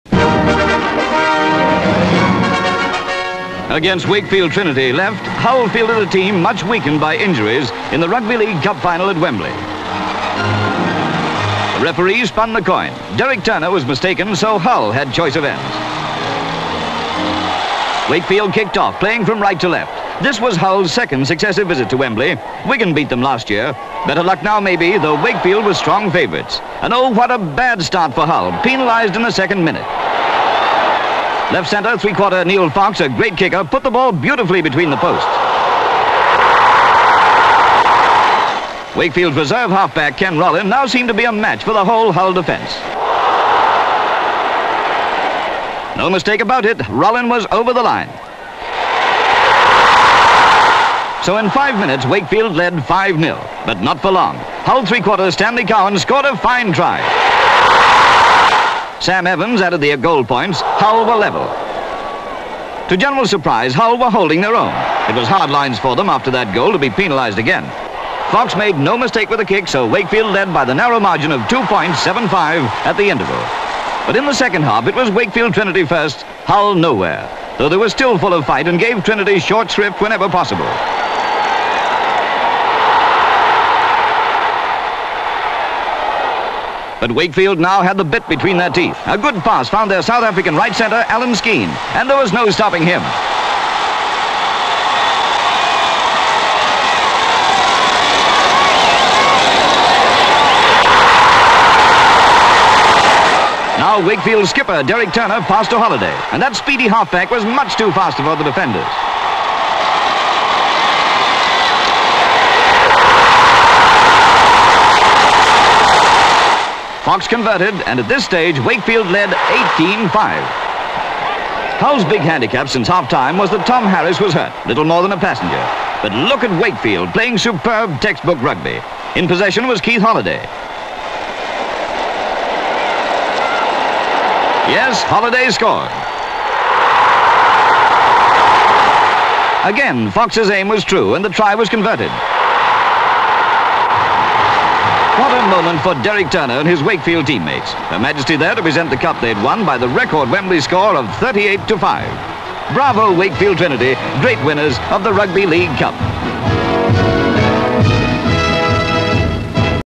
Rugby League Cup Final (1960)
Item title reads - Rugby League Cup Final.

Wakefield beat Hull 38 - 5. Wembley, London.

M/S as the two teams walk out onto the pitch, Wakefield are in white shorts.  L/S huge crowds. Various shots as the two captains, Whiteley of Hull and Derek Turner of Wakefield, toss the coin which Whiteley wins.  Various shots of the match in progress, a Hull man is brought down. C/U people in crowd.   M/S scrum, Hull is penalised for an infringement.  C/U man in crowd shouting. M/S Neil Fox running up to take penalty for Wakefield, he kicks, ball goes between posts to give Wakefield a 3 - Nil lead. C/U boy in crowd shouting. Various shots of Ken Rollin in possession for Wakefield, he runs through mass of players and around Hull defender, he gathers the ball again near the touchline and goes over for a try.  L/S crowd shouting. M/S scrum, the ball goes to Hull player Stanley Cowan who evades a tackle and runs on to go over the line for a try. L/S massed crowd. M/S Sam Evans taking converting kick, ball goes between the posts to make score 5 - 5. C/U woman in crowd. M/S's with Wakefield in possession, a man is brought down. C/U woman in crowd. M/S Fox taking kick for Wakefield, the ball goes between the posts to give Wakefield the lead 7 - 5.  L/S of crowds at half time.

Various shots of play in second half. Turner passes to Alan Skene who touches down for Wakefield. The crowds cheer. Derek Turner passes to Keith Holliday who goes over the line. Fox takes converting kick and scores - Wakefield 18 Hull 5. C/U small boy cheering. Ball is passes from Skene to Firth, Rollin, Chamberlain and Holliday who touches down. C/U man in crowd shouting and sinking back into his seat dejectedly.  Fox scores again making final score Wakefield 38- Hull 5. L/S crowd applauding.  M/S Turner leading his team up to receive the Cup from Queen Elizabeth II. M/S as Turner receives the Cup from the Queen. M/S Turner with Cup. L/S crowd.  Elevated shot of the Wakefield team chairing Turner around the pitch.
 FILM ID:1680.1